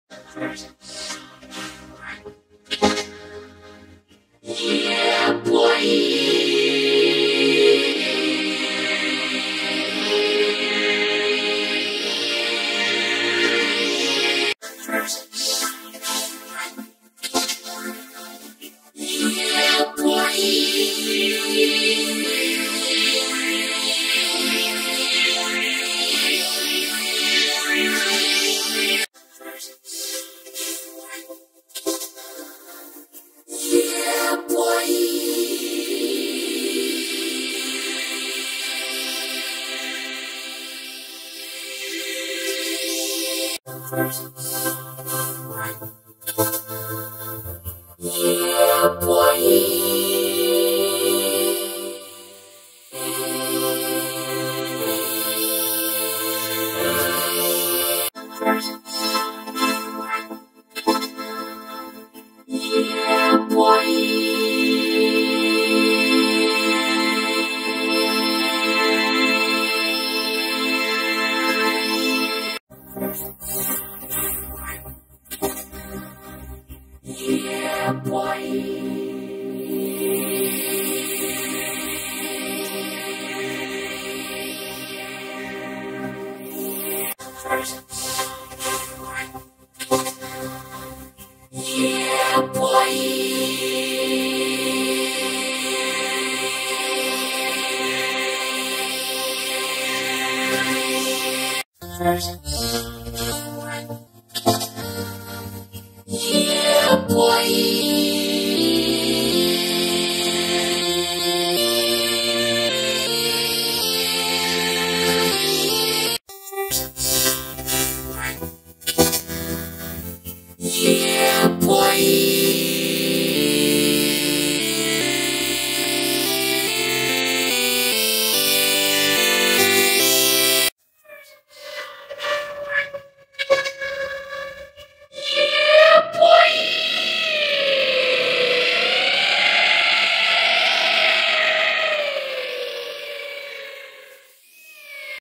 Yeah Boy - Vocoded in 11 different ways
The famous Yeah Boy video now vocoded and with added content aware video effect.